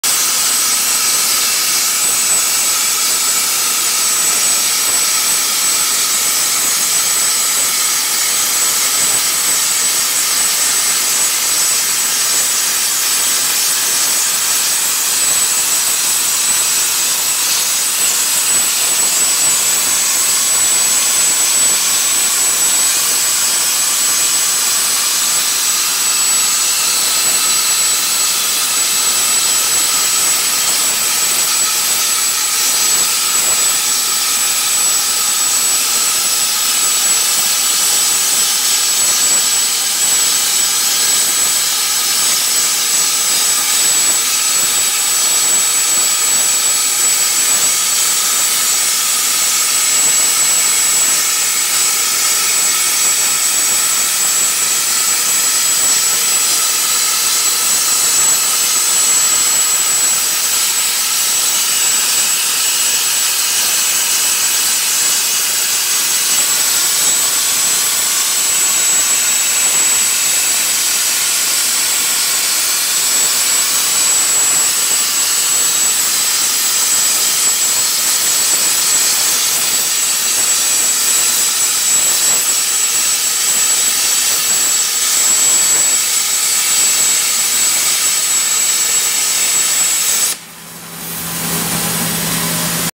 5 axis plasma cutting machine
New cutting video by our 5 axis plasma cutting machine. 5 axis plasma cutting machine is used for all kinds of thick metal.